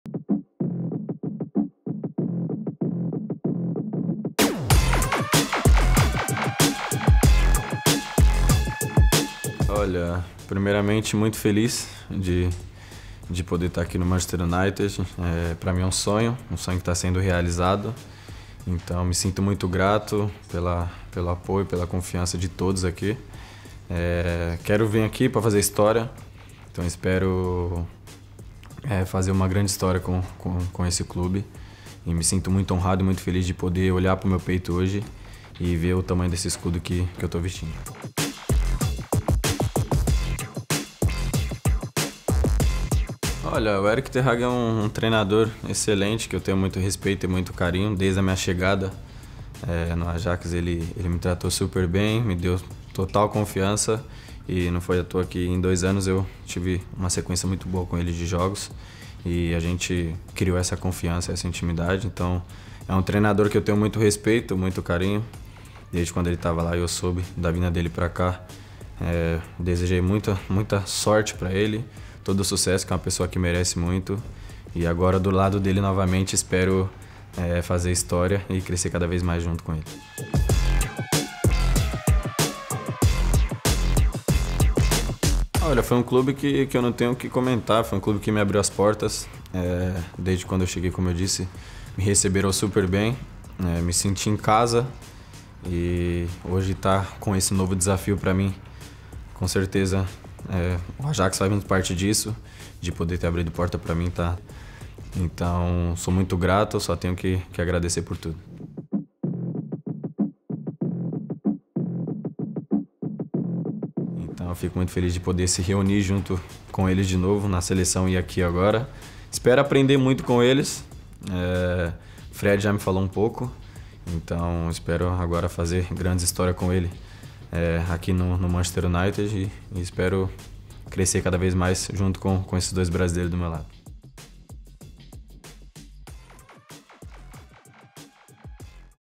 "It's my DREAM come true! 🔴 | Man Utd's new signing Antony
Antony speaks to MUTV for the first time after completing his £85.4m move from Ajax...